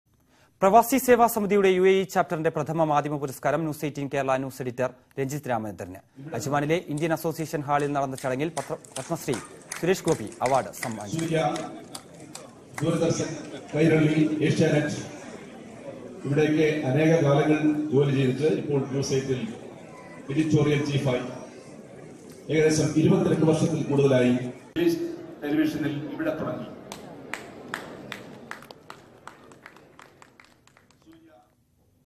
പ്രവാസി സേവാസമിതി UAE ചാപ്റ്ററിന്റെ പ്രഥമ മാധ്യമ പുരസ്കാരം Renjith Ramachandranന് | Kerala News
പ്രവാസി സേവാസമിതി UAE ചാപ്റ്ററിന്റെ പ്രഥമ മാധ്യമ പുരസ്കാരം Renjith Ramachandranന്. News18 kerala News Editor ആണ് രഞ്ജിത്ത് രാമചന്ദ്രൻ. പുരസ്കാരം നടൻ Suresh Gopi സമ്മാനിച്ചു. Indian Association Hall Ajmanലാണ് ചടങ്ങ് നടന്നത്.

About the Channel:
| n18_Ad_Present